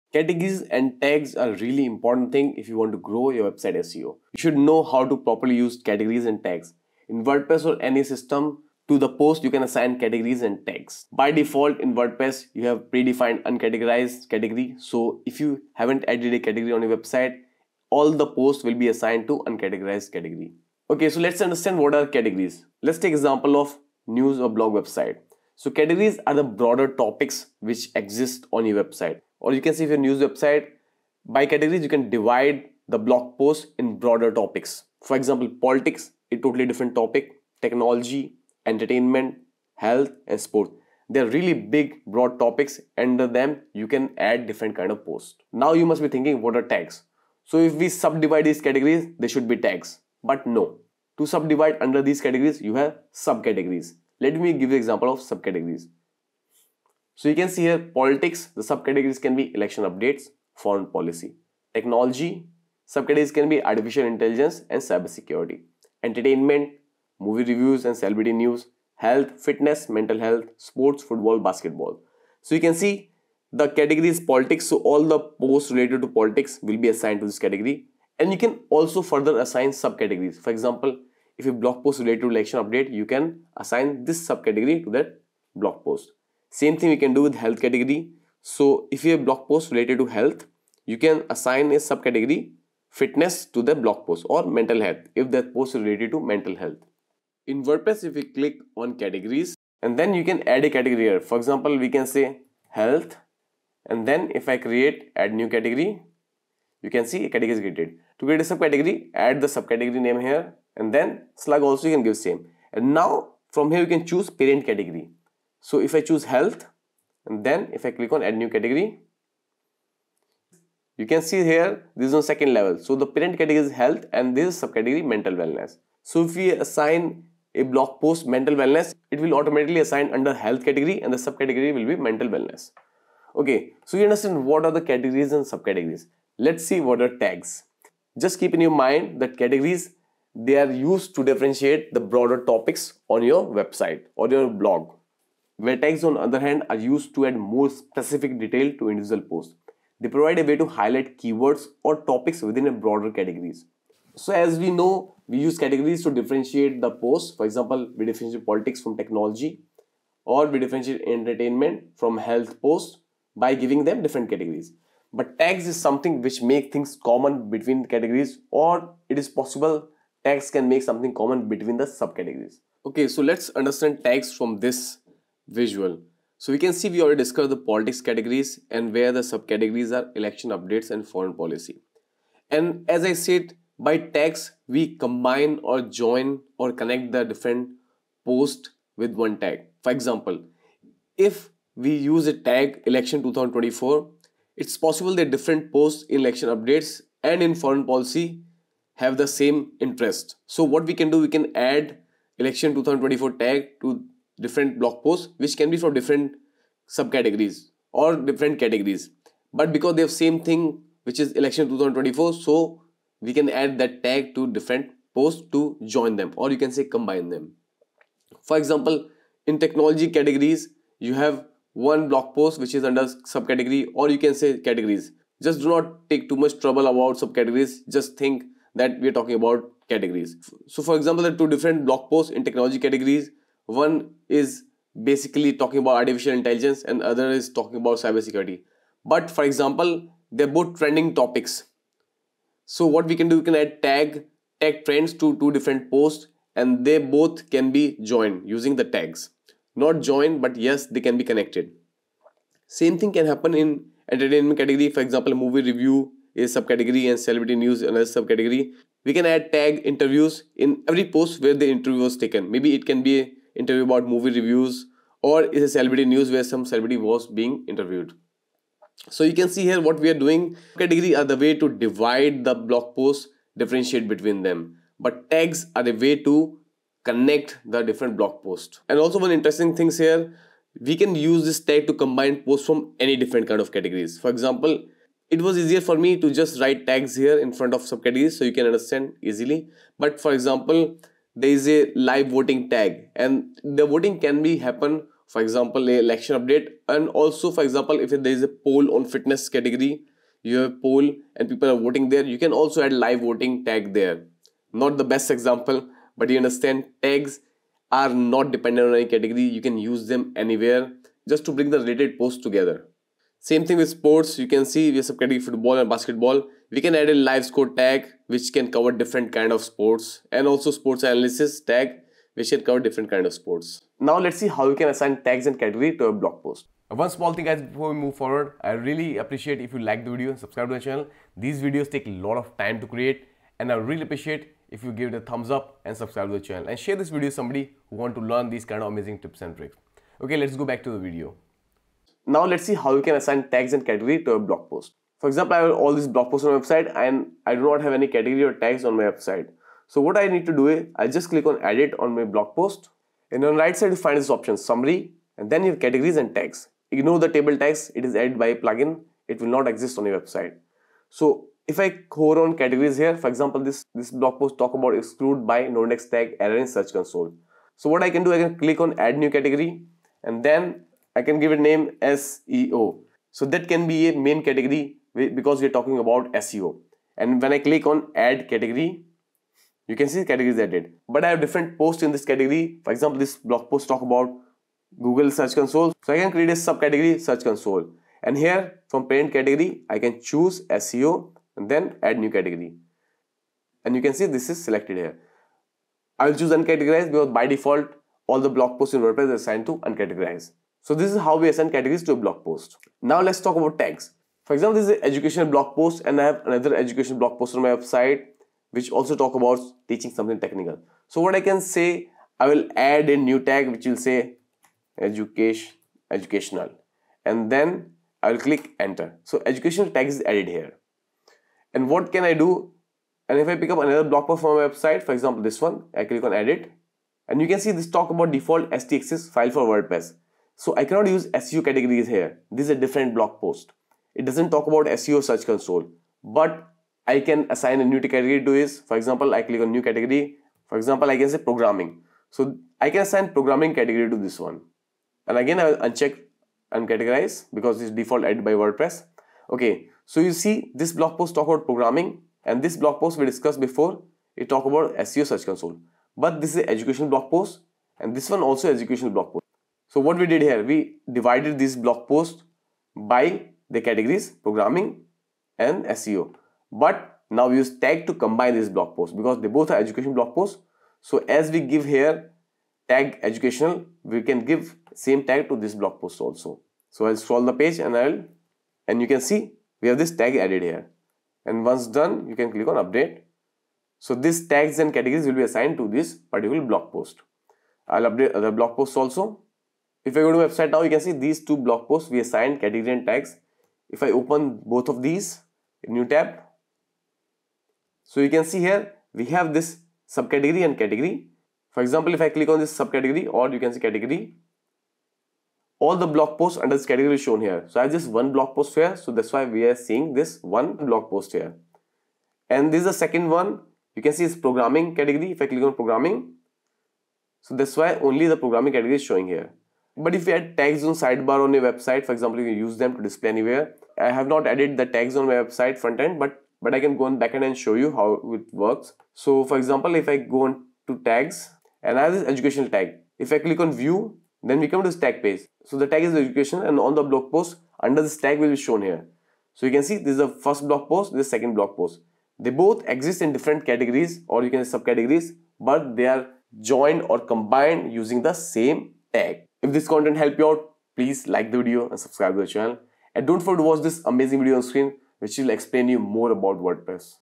How to Use Categories & Tags in WordPress - Important SEO Tutorial
Rank higher in google and Index your pages quickly by setting up Categories and Tags properly.

In WordPress, categories and tags are two taxonomies used to organize and classify content. While they serve similar purposes, there are key differences in their use and functionality.

In summary, categories provide a broad organizational structure, while tags offer a more granular and flexible way to associate posts with specific topics or keywords. Together, they enhance the user experience by making it easier for visitors to find and explore content that aligns with their interests.

Categories
Categories are used to group content into broad topics or sections. They provide a way to organize and structure the main themes of your website.

    Politics
    Technology
    Entertainment
    Health
    Sports

Tags, on the other hand, are used to add more specific details to individual posts. They provide a way to highlight keywords or topics within the broader categories.

    Politics Category:
        Election2024
        ForeignPolicy

    Technology Category:
        Gadgets

    Entertainment Category:
        MovieReviews
        CelebrityNews

    Health Category:
        FitnessTips
        MentalHealth

    Sports Category:
        Football
        Basketball
        Olympics2024